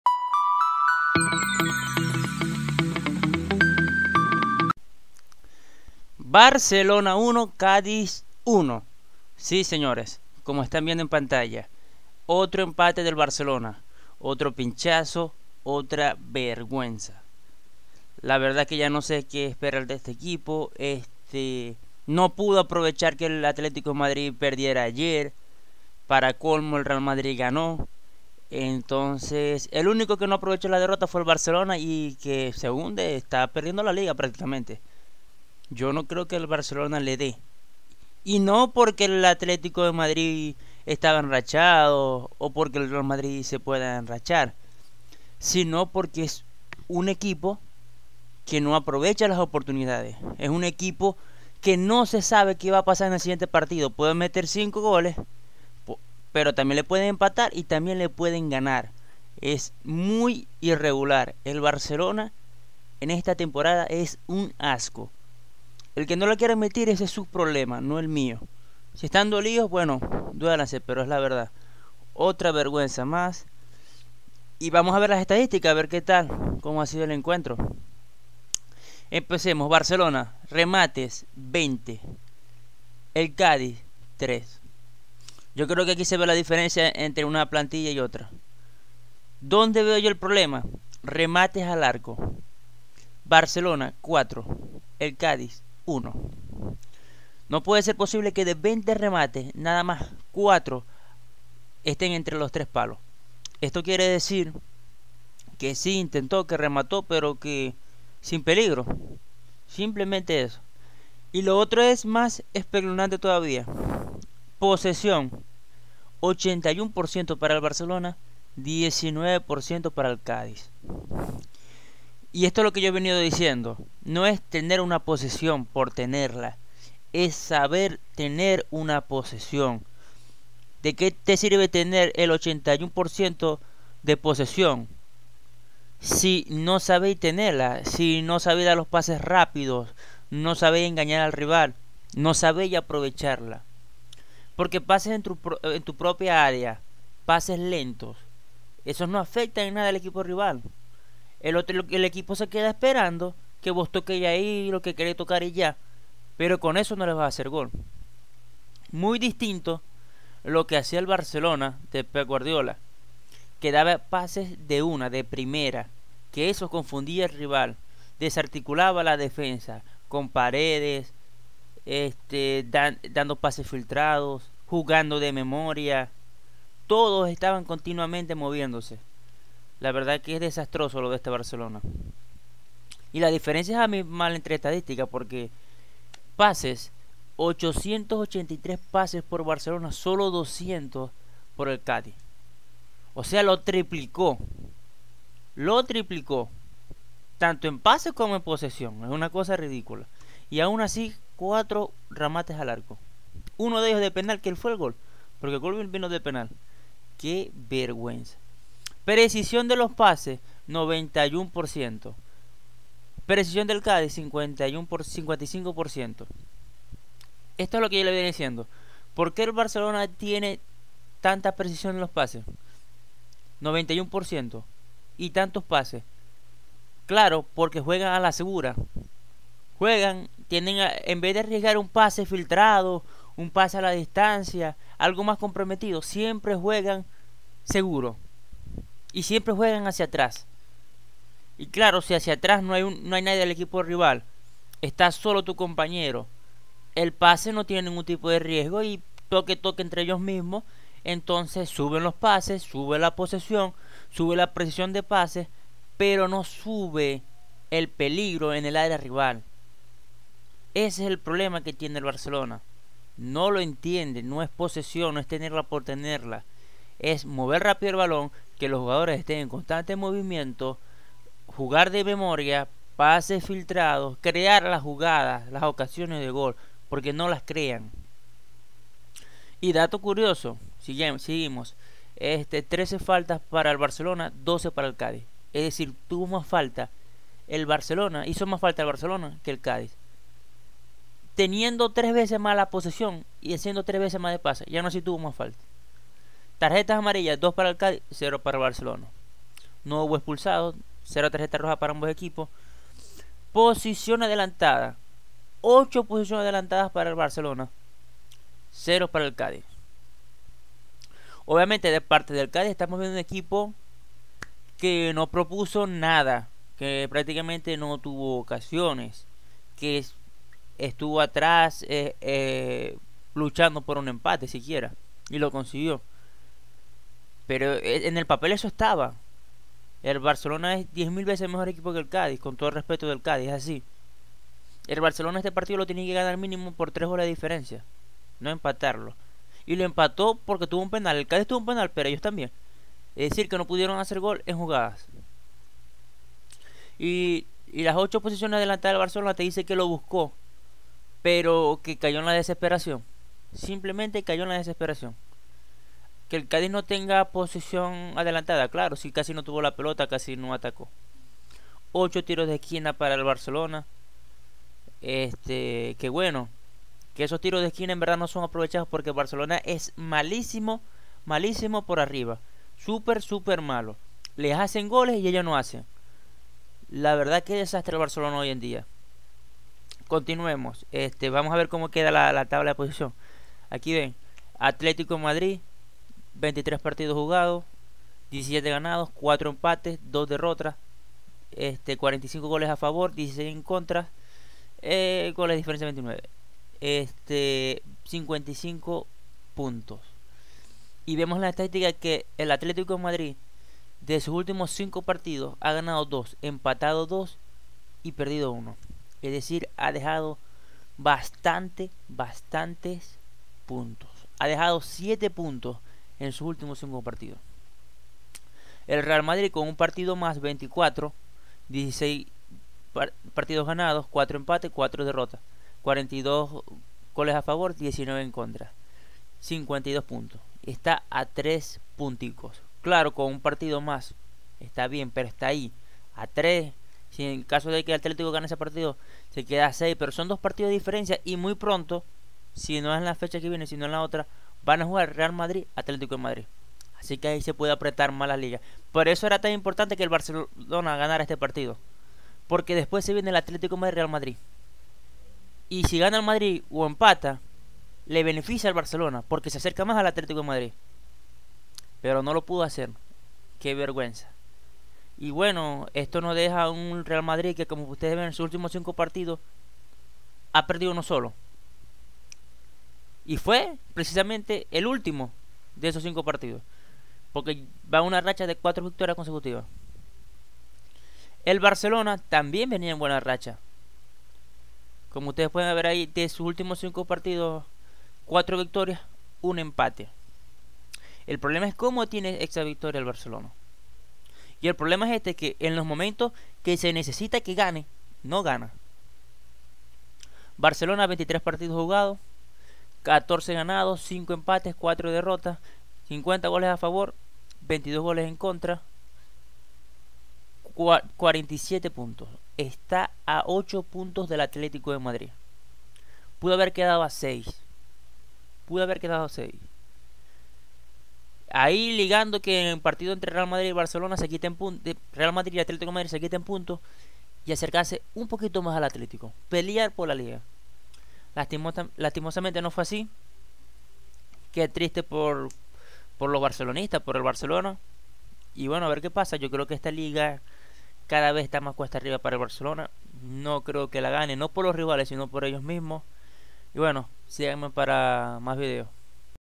BARCELONA TIRA LA LIGA EMPATANDO CONTRA EL CADIZ
81% DE POSESION DE BALON, MAS DE 20 REMATES Y EL GOL LO HACEN ES DE PENALTI!
QUE VERGUENZA, SE NECESITA OTRO ENTRENADOR